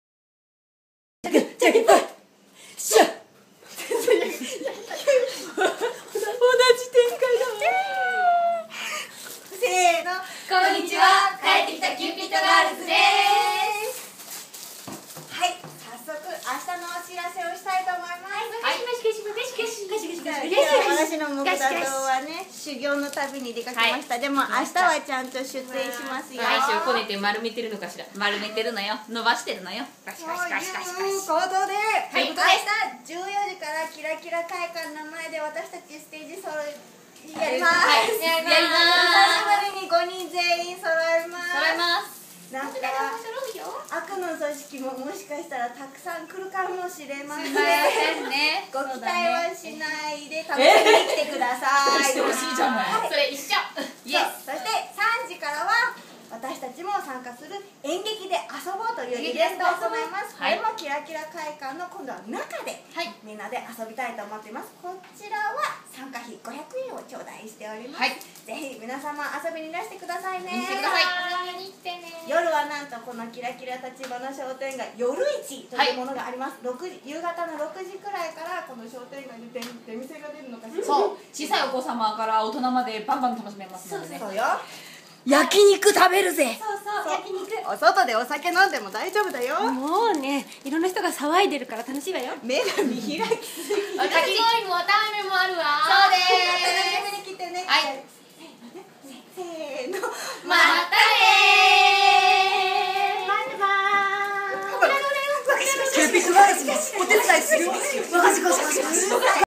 帰ってきたキューピッドガールズ0907
帰ってきたキューピッドガールズです。
明日は年に一度のキラキラ橘商店街の夜市！
私たちは14時からステージ。夜までどーんと楽しんで行ってくださいね☆お待ちしております！